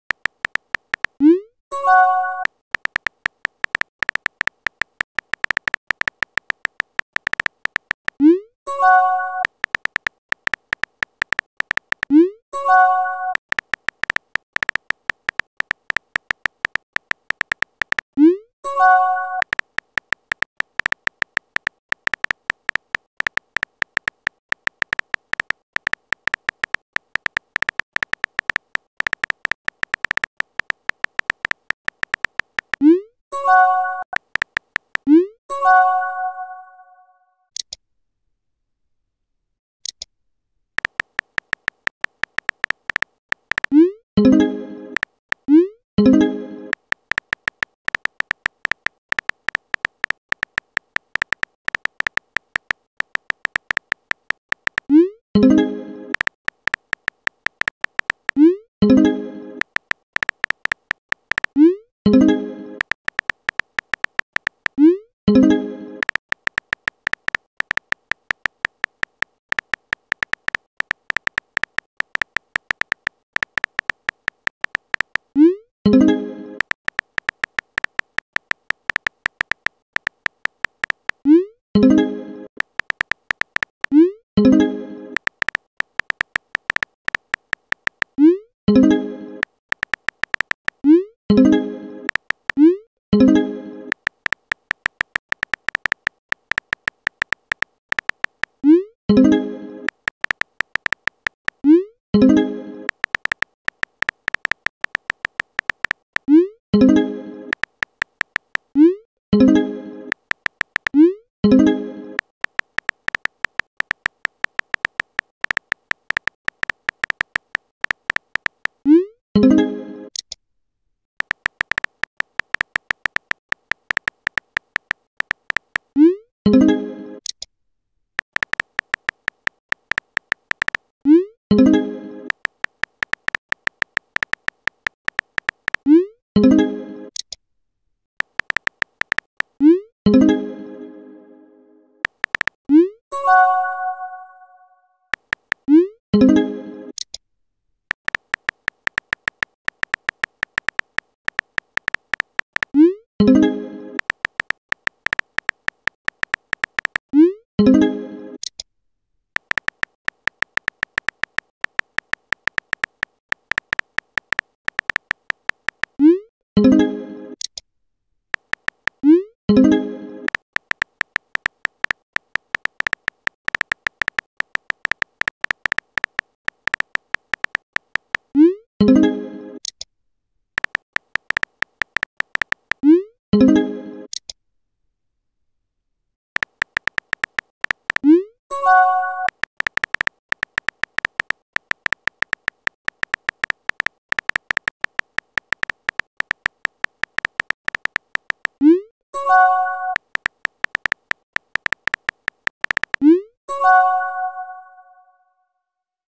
Texting Story #7: Edward's reaction to Emily's Best Friend
So, textingstory, this is about Edward. His react to Emily's Best Friend. After he watch it. Edward really out of his mind. I hope you like it.